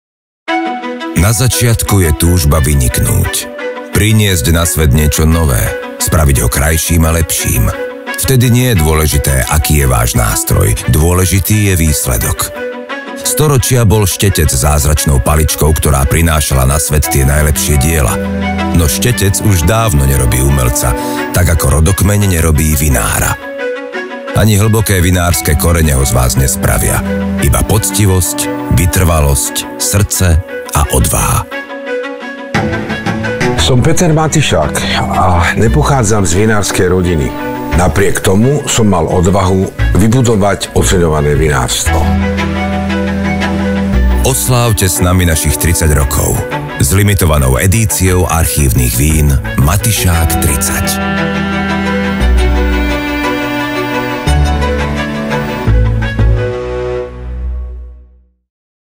Štetec nerobí umelca, rodokmeň vinára
Už 30 rokov dokazujeme, že predsudky nerobia vinára. Pri príležitosti výročia nášho vinárstva sme v spolupráci so slovenskými umelcami pripravili limitovanú edíciu vín. Oslávte to s nami.

CABERNET SAUVIGNON ROSÉ 2015 - limitovaná edícia 1000 fliaš
Etiketa: Marek Cina

FRANKOVKA MODRÁ 2012 - limitovaná edícia 850 fliaš
Etiketa: Dávid Demjanovič & Jarmila Mitríková

RIZLING RÝNSKY 2017 - limitovaná edícia 2200 fliaš
Etiketa: Martin Schwarz